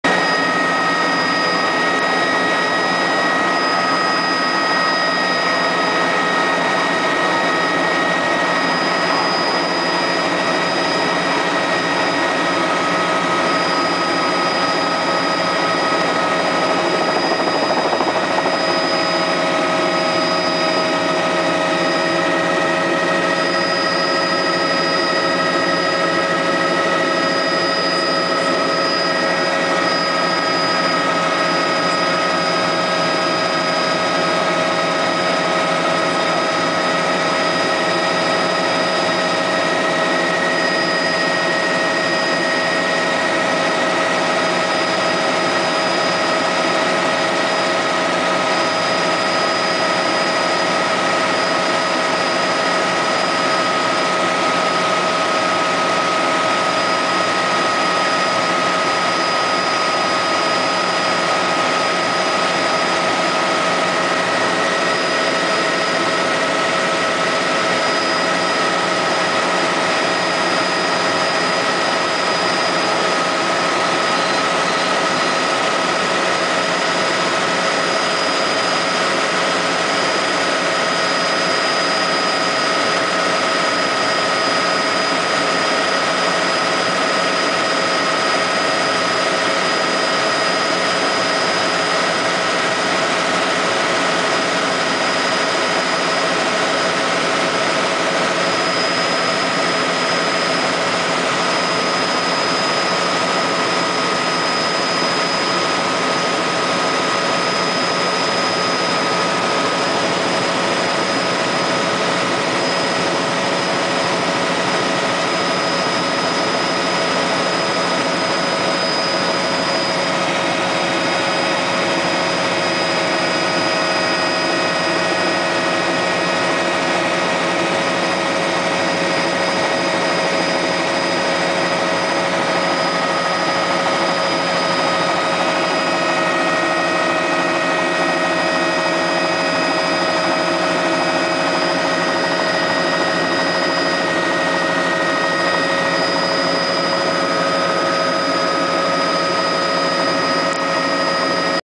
Helicoptering into fog
we fly into fog then go above it, awesome day! love my job!